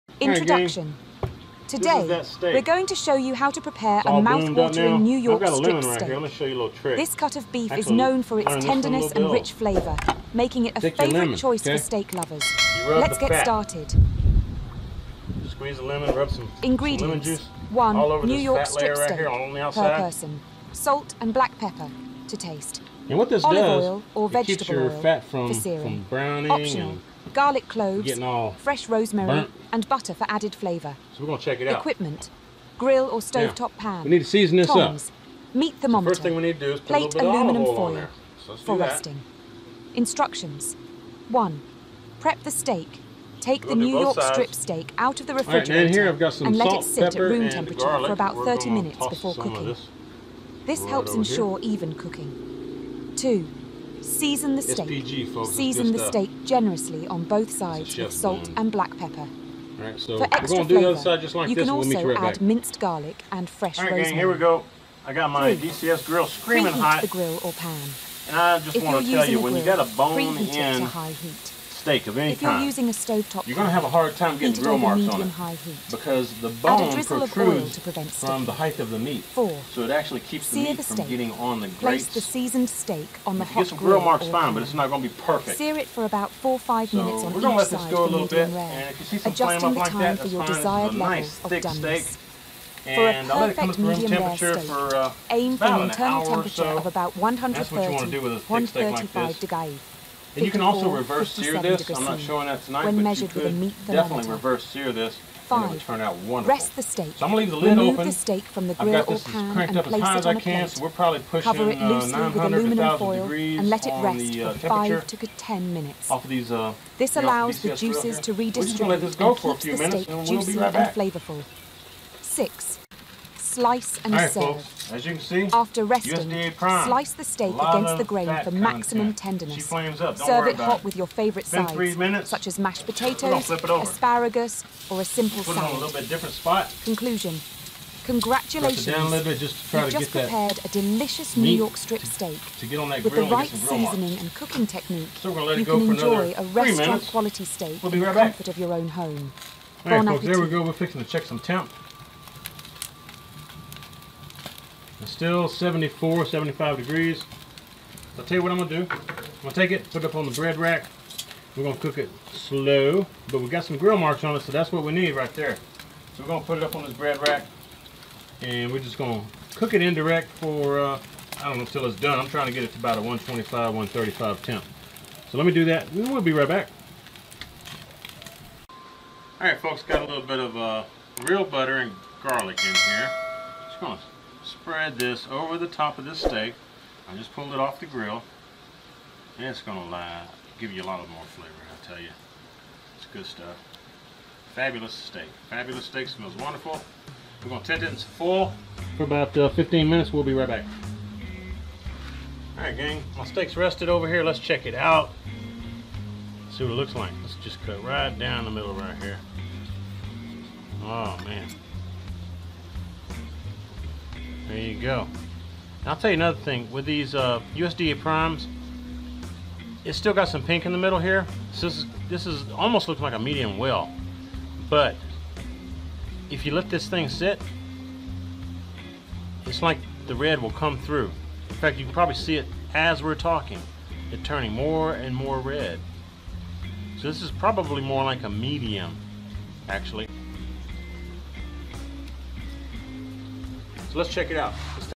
Cooking the Perfect New York Strip Steak: Tips and Techniques
Introduction:

"Today, we're going to master the art of grilling the perfect New York Strip Steak. With the right techniques, you can achieve that beautiful sear, tender interior, and mouthwatering flavor. Let's dive into the world of grilling excellence!"

Ingredients:

New York Strip Steaks (1 per person)
Salt and black pepper (to taste)
Olive oil or vegetable oil
Optional: Garlic cloves and fresh rosemary for extra flavor
Equipment:

Grill
Tongs
Meat thermometer
Plate
Aluminum foil (for resting)
Instructions:

1. Prepare the Grill:

Start by preheating your grill to high heat. Make sure the grates are clean and lightly oiled to prevent sticking.
2. Season the Steaks:

Take your New York Strip Steaks and season them generously on both sides with salt and black pepper. For added flavor, consider minced garlic and fresh rosemary.
3. Oil the Steaks:

Drizzle a bit of olive oil or vegetable oil over both sides of the steaks. This helps with the searing process.
4. Grill the Steaks:

Place the steaks on the hot grill grates. For a perfect medium-rare steak, grill for about 4-5 minutes on each side. Adjust the time according to your preferred doneness.
Use a meat thermometer to ensure accuracy. Aim for an internal temperature of around 130-135°F (54-57°C) for medium-rare.
5. Rest the Steaks:

Once your steaks are done grilling, transfer them to a plate and cover them loosely with aluminum foil. Let them rest for 5-10 minutes. This allows the juices to redistribute, resulting in a tender and juicy steak.
6. Slice and Serve:

After resting, slice the steaks against the grain for maximum tenderness. Plate them up and serve alongside your favorite sides, whether it's grilled vegetables, baked potatoes, or a fresh salad.
Conclusion:

"Congratulations, you've just grilled the best New York Strip Steak! With the right preparation and attention to detail, you can enjoy a restaurant-quality steak in your own backyard. So fire up that grill and savor the deliciousness. Happy grilling!"

Enjoy your perfectly grilled New York Strip Steak!